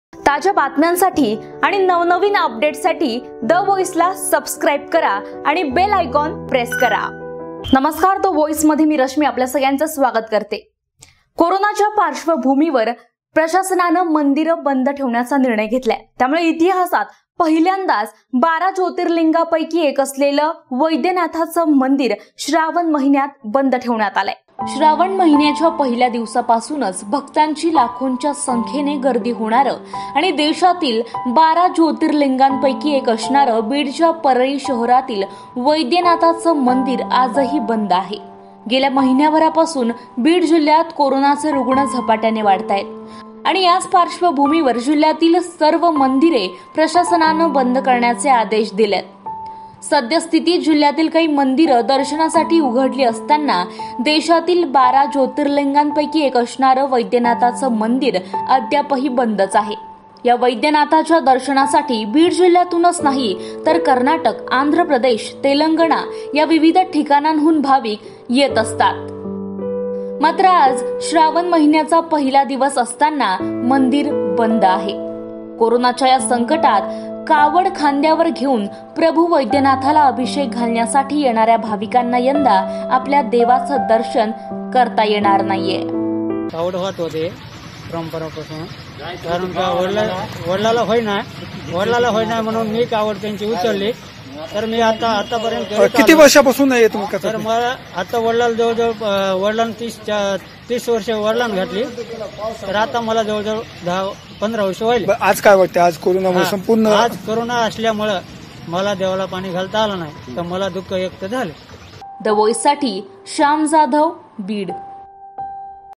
Beed-बारा ज्योतिर्लिंगांपैकी एक असणारे वैद्यनाथाचे मंदिर श्रावण महिन्यात बंद |The Voice News Channel
The Voice News Marathi
TheVoiceNewsChannel
TheVoiceNewsMarathi
thevoicenewschannel
thevoicenewsmarathi